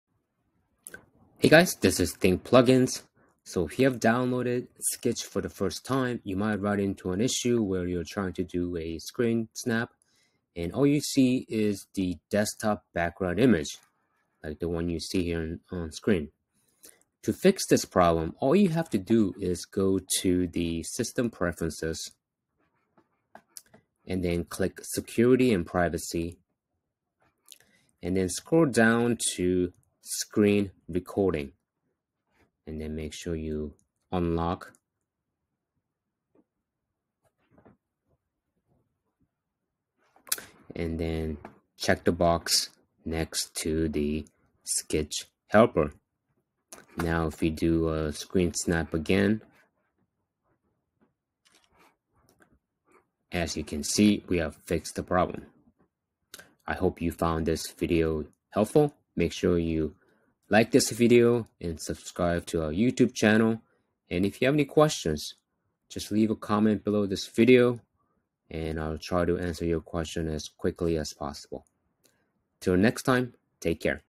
Skitch Only Captures Desktop - Fix
Skitch not working on mac. Skitch taking screenshots of desktop? See the video for the solution.